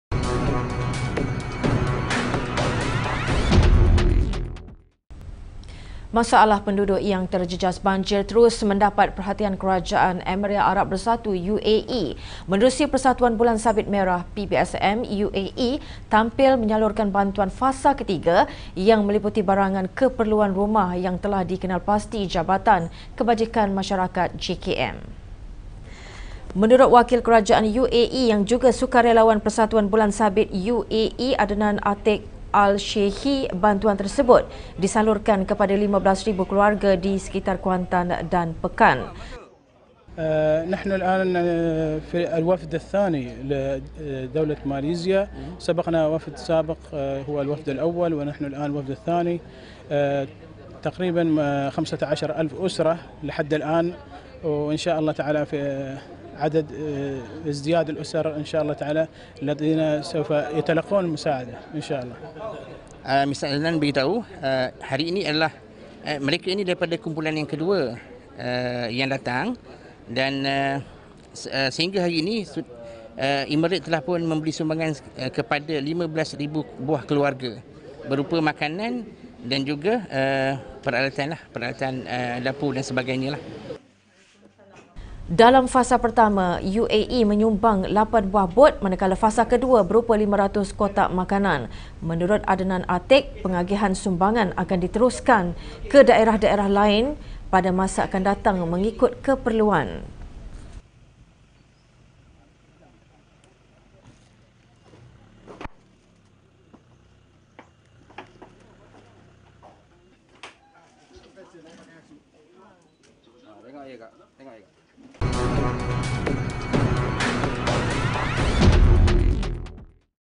UAE Salur Bantuan Kepada Penduduk Terjejas Banjir
Masalah penduduk yang terjejas banjir terus mendapat perhatian kerajaan Emiriyah Arab Bersatu (UAE).

Menerusi Persatuan Bulan Sabit Merah, PBSM, UAE tampil menyalurkan bantuan fasa ketiga yang meliputi barangan keperluan rumah yang telah dikenalpasti Jabatan Kebajikan Masyarakat (JKM).